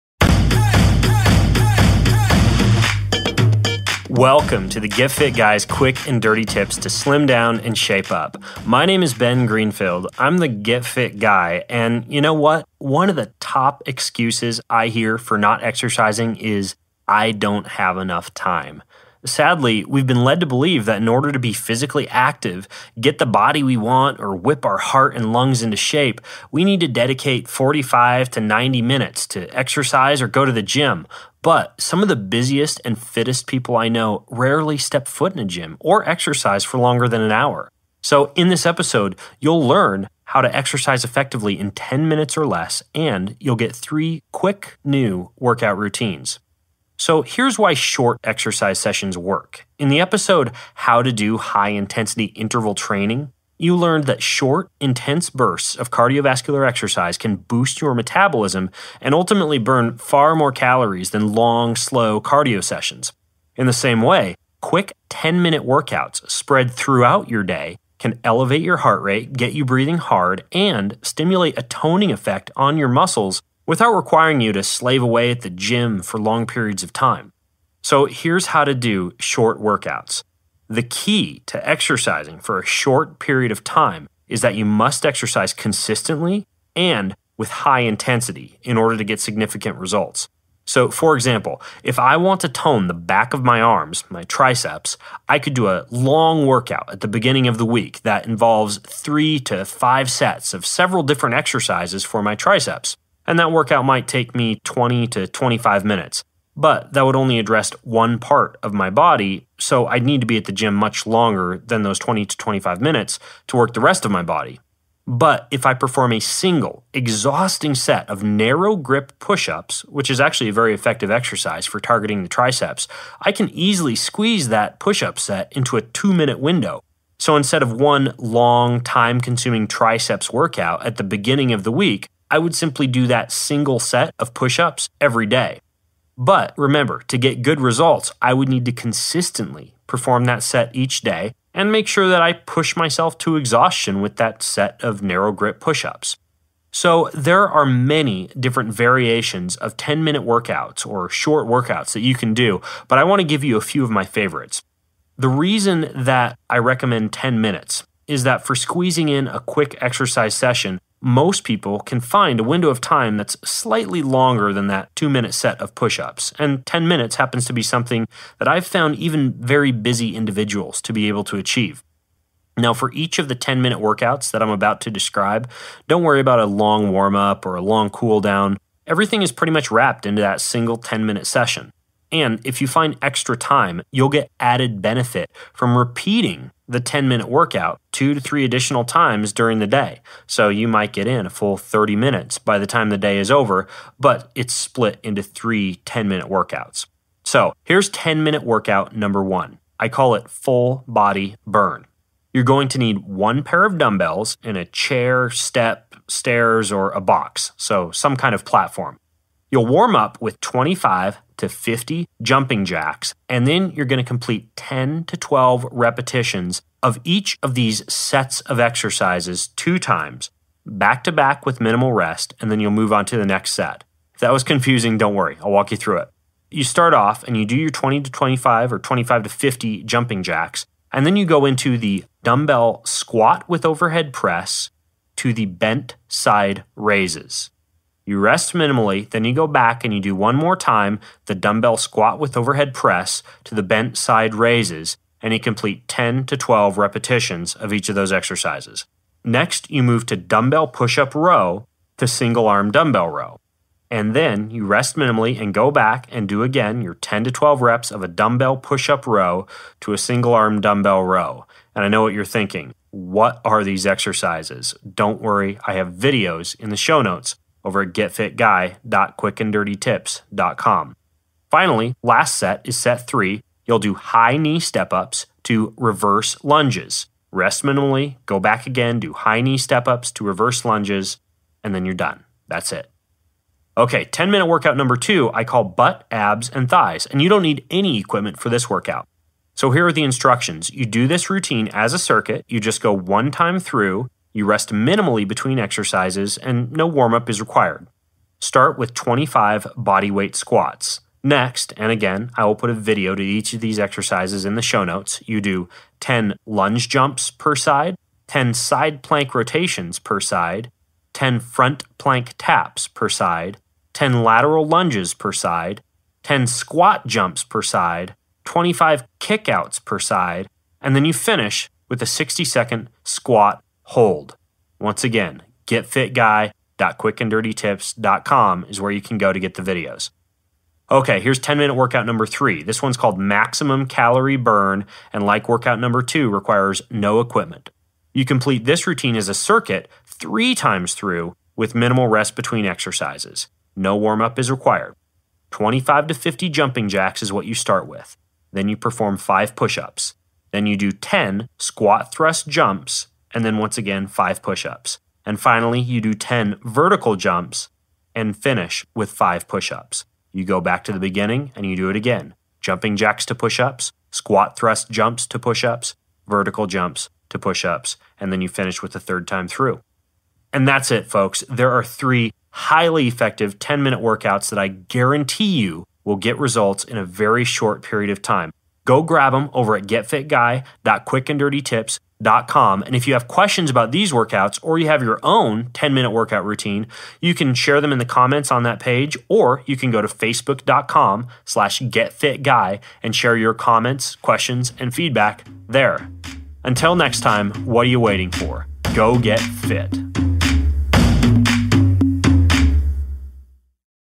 058 GFG How to Do a 10-Minute Workout
Learn how to exercise effectively in 10 minutes or less, and get 3 quick new workout routines. For shownotes, a free newsletter, and more, go